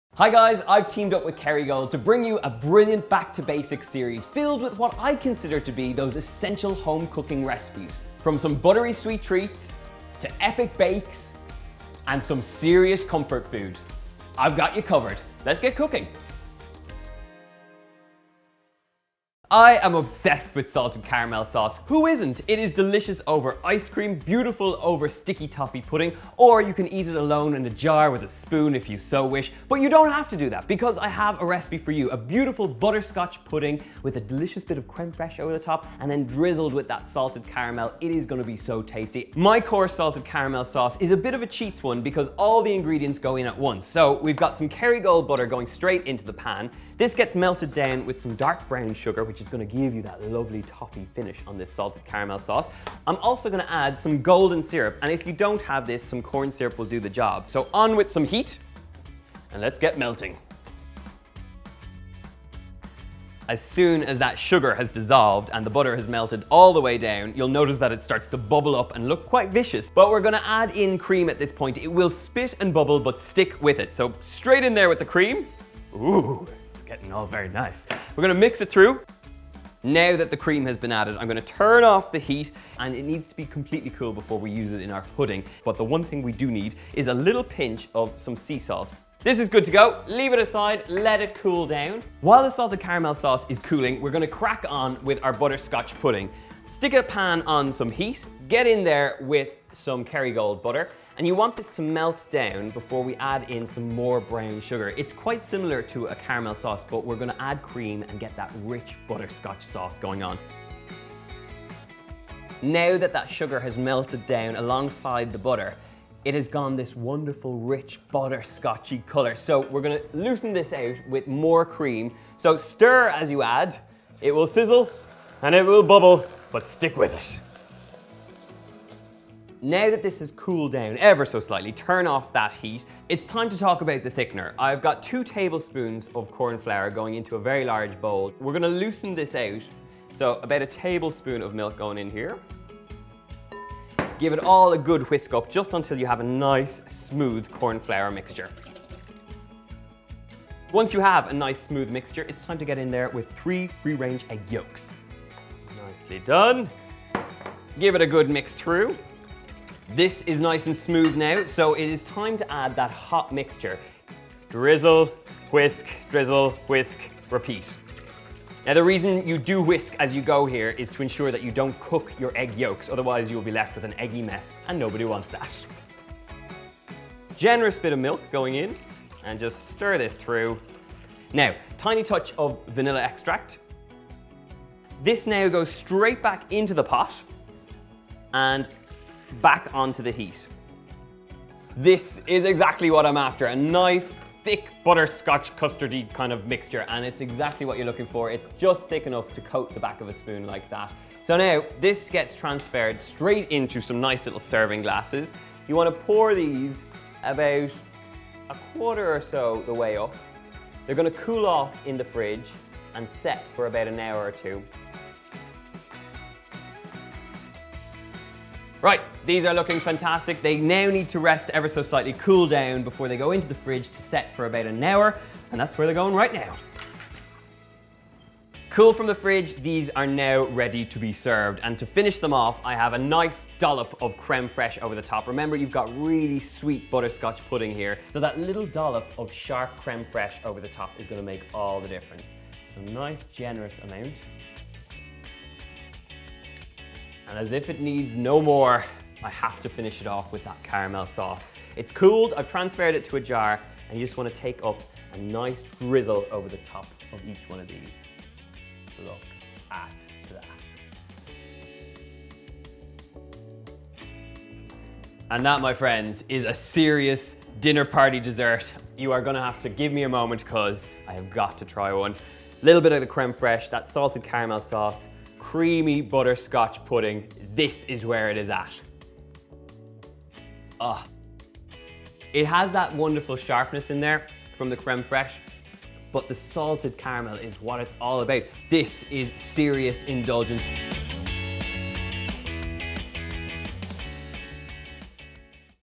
Butterscotch Pudding with Salted Caramel | Back to Basics | Donal Skehan Kerrygold Recipes
Kerrygold teamed up with Donal Skehan to go Back to Basics and rediscover delicious classic recipes.
Butterscotch Pudding is a perfect dinner party dessert; and if you're crazy about salted caramel, you'll love this treat!